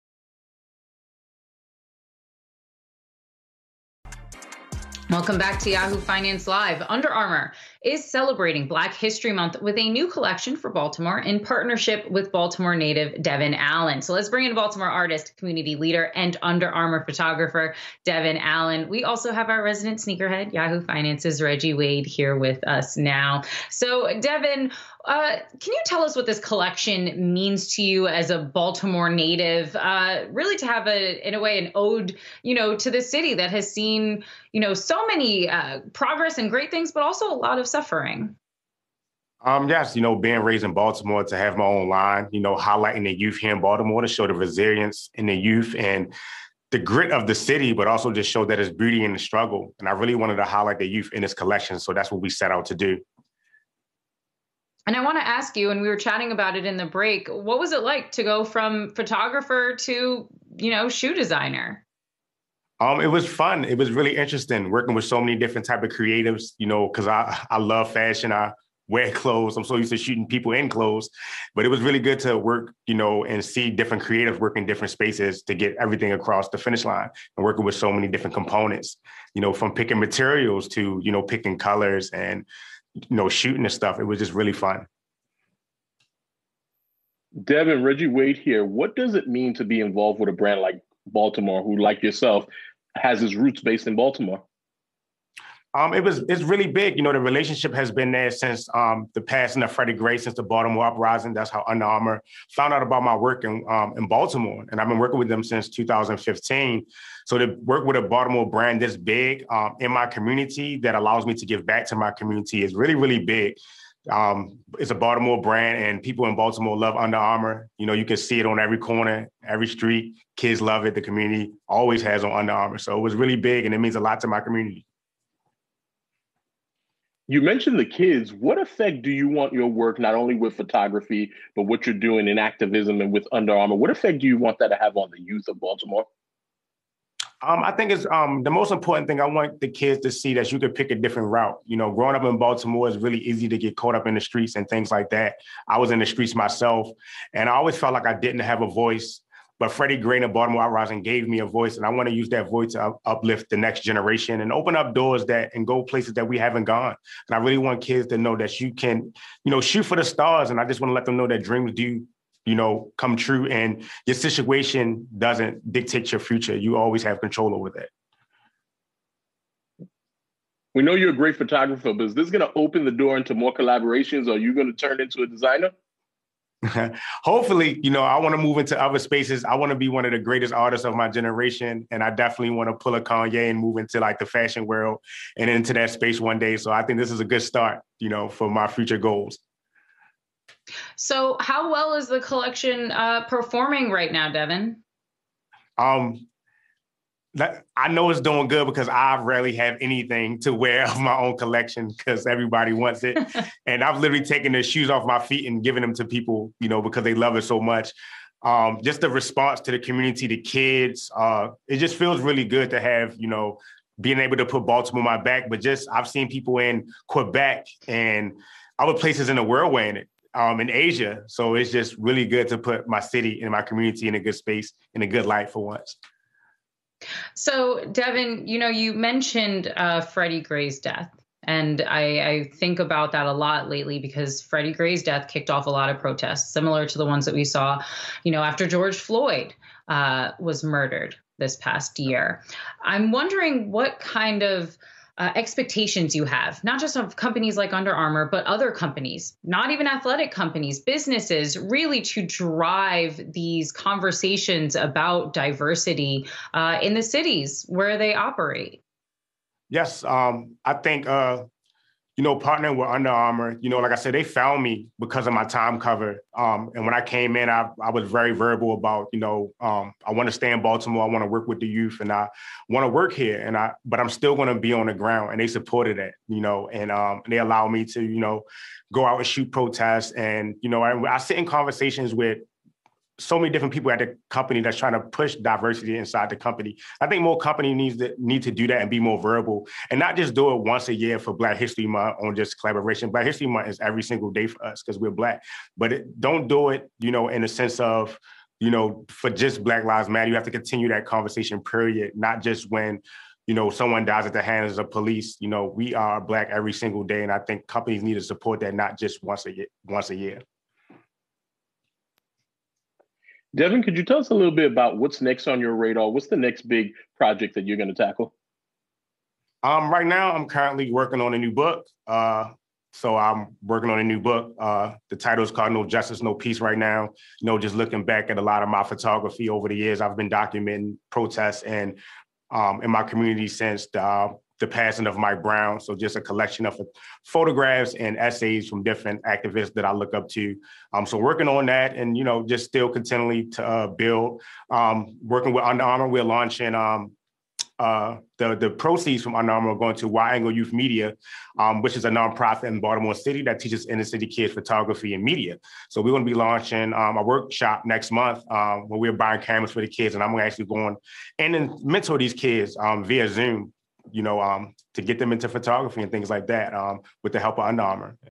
Under Armour photographer Devin Allen drops special BHM collection
Devin Allen, Baltimore Artist, Community Leader, and Under Armour Photographer, joins Yahoo Finance's Kristin Myers and Reggie Wade to discuss his new clothing collection.